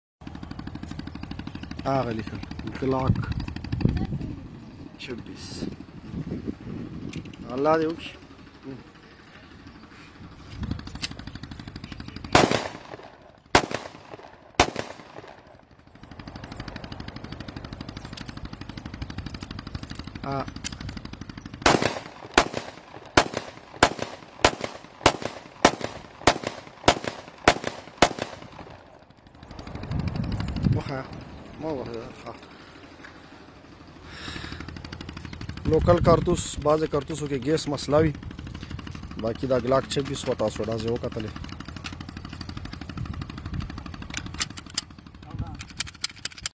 Glock 17 | Transparent Body Fully Automatic
Fully Automatic Glock 17 pistol with transparent body